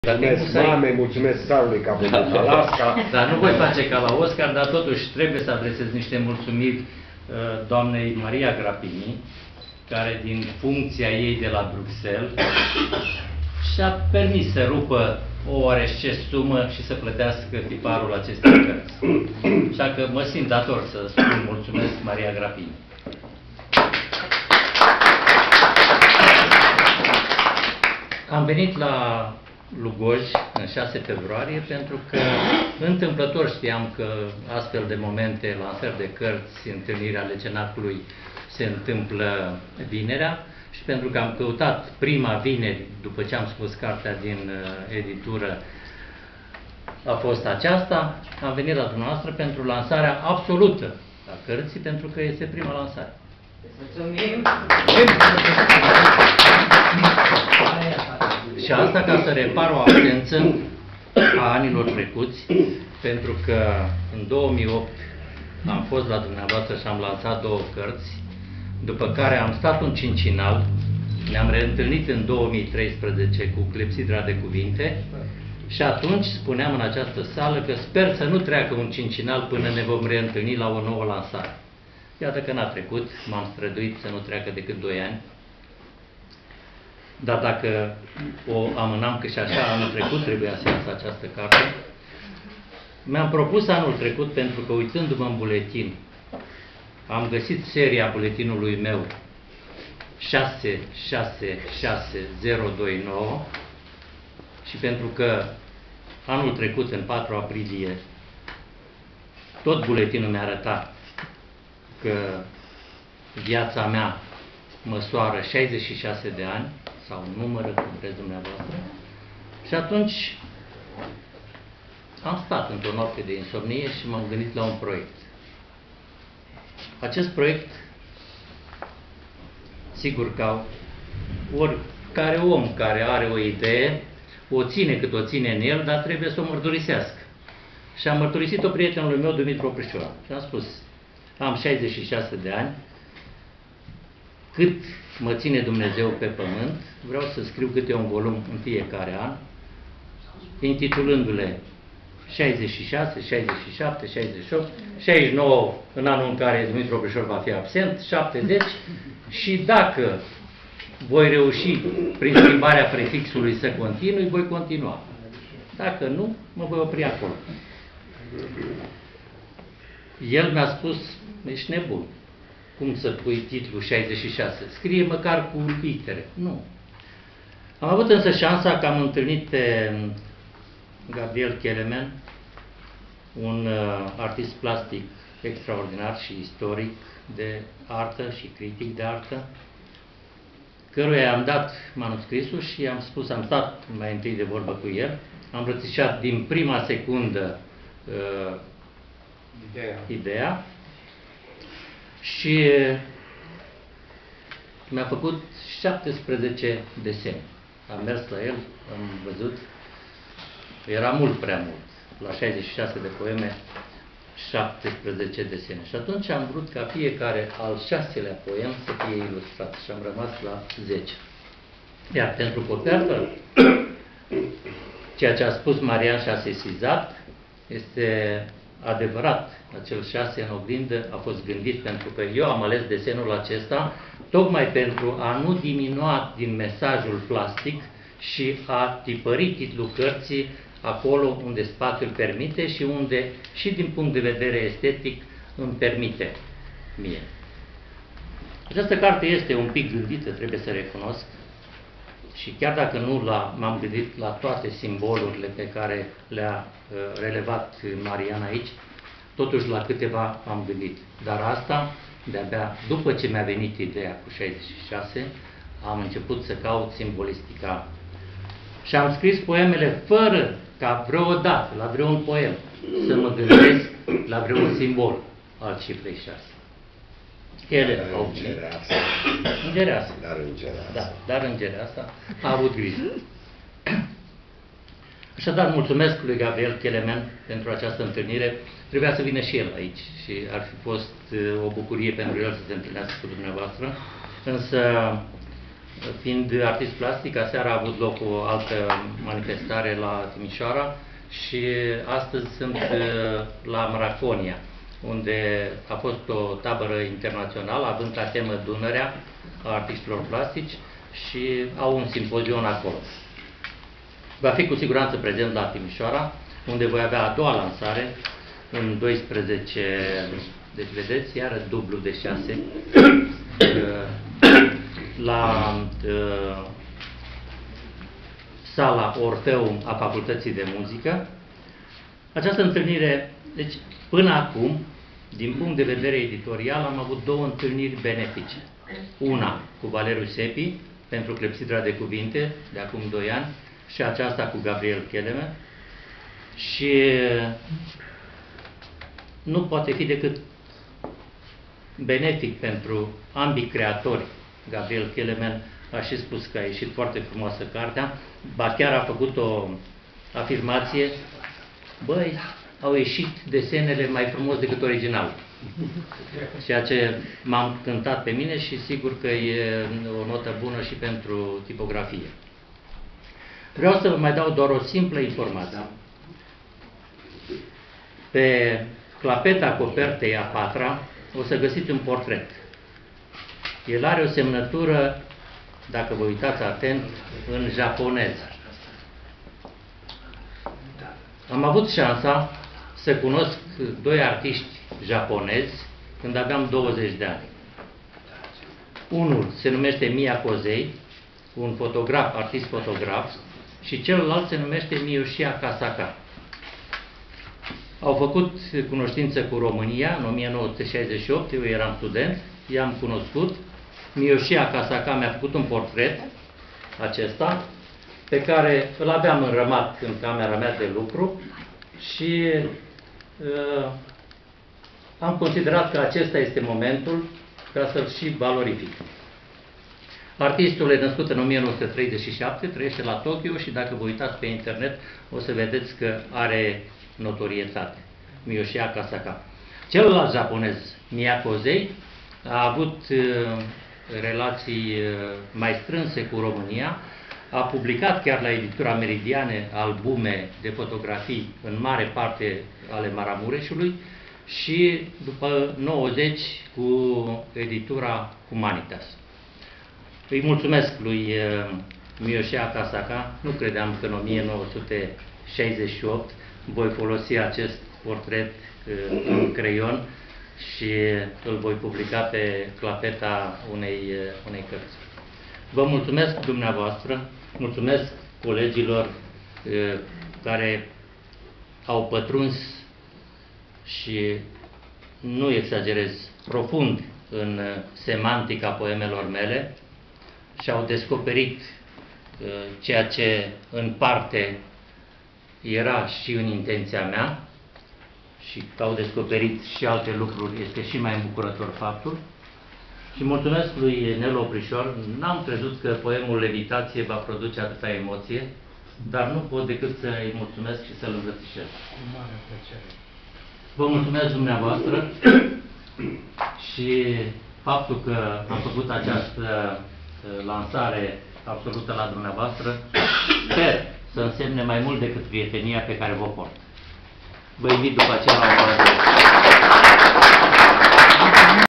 George Schinteie - lansarea volumului 66 - Lugoj - 6 02 2015 -video4 Adriana Weimer
George Schinteie - lansarea volumului de poezie ,,66,, - au vorbit: George Schinteie, Marian Odangiu, George Popovici, Dumitru Oprisor, Remus Valeriu Giorgioni, Ion Oprisor, Nicolae Streian - recital folk sustinut de cantautorul George Popovici - organizatori: Casa de Cultura a Municipiului Lugoj & Cenaclul Anotimpuri Lugoj - 6 februarie 2015 - video 1, 2, 3, 4, 5, 6 de Adriana Weimer